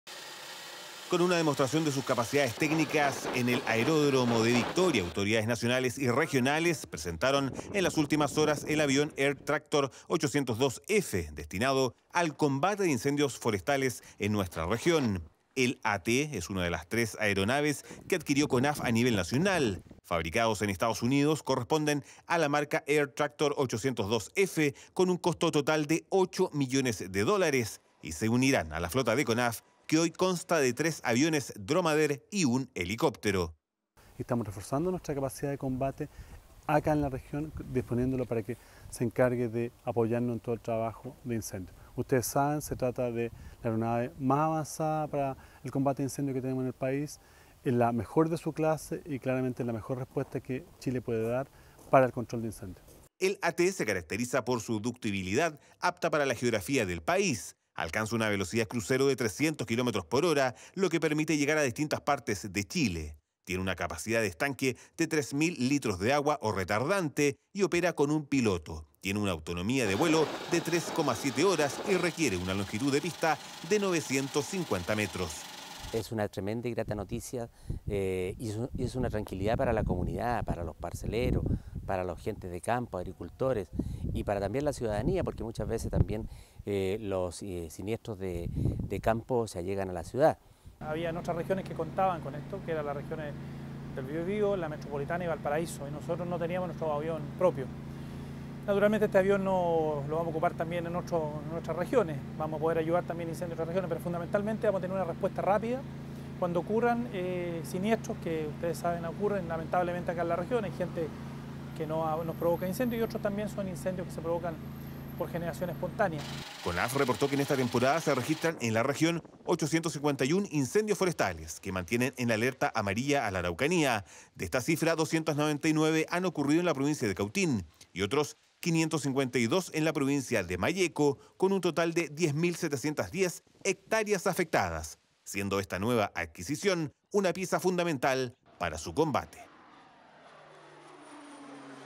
Nuevo avión para Conaf para combatir indendios forestales en La Araucanía | UFROVISIÓN
En la comuna de Victoria, fue presentada la nueva adquisición de CONAF.
Se trata de un avión de alta tecnología y versatilidad, con una capacidad de estanque de 3 mil litros de agua. Una nueva herramienta de apoyo, para el combate efectivo de incendios forestales en nuestro país.